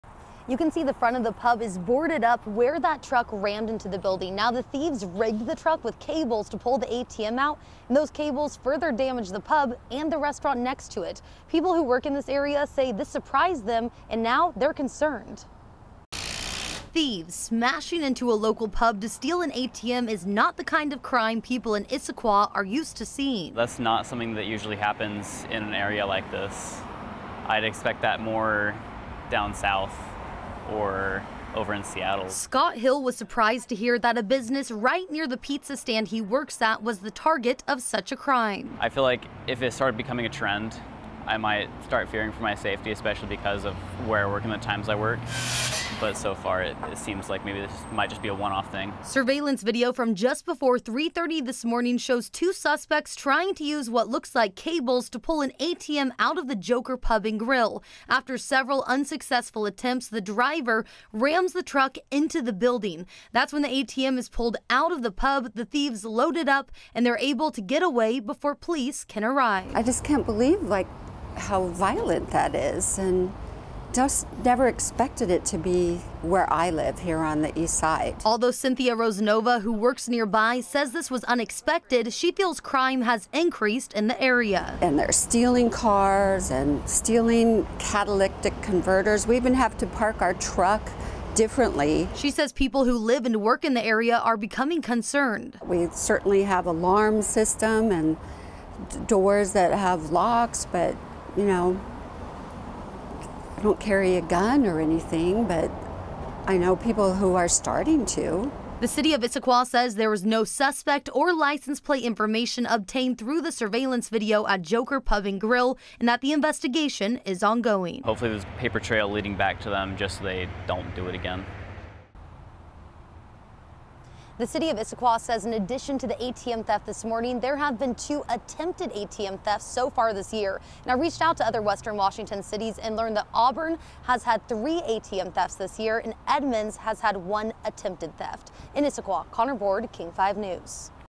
Issaquah residents shocked after ATM theft from local pub
A group of suspects rammed a car into an Issaquah pub early Thursday morning and stole an ATM from inside the business.

Just before 3:30 a.m. Thursday, surveillance footage shows people attempting to extract the ATM from Joker Pub and Grill off East Lake Sammamish Parkway SE. Two of the suspects appear to be trying to tie something to the ATM and then a third person in a blue truck trying to drive away and pull the machine out.